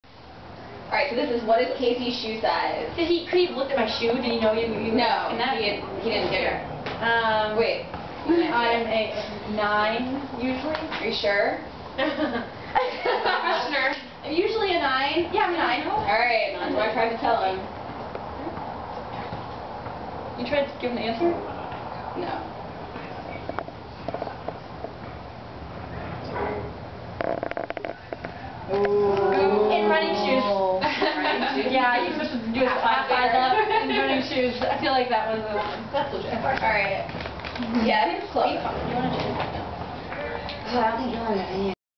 Time for a Bridal Shower
Bridal shower for Casey at Maggie's home. Hosted by Maggie, Lisa and Allison. Time themed shower. Joe on video and Casey answering questions in a "Newly Wed" game. Sunday, October 14, 2012  2:00 p.m.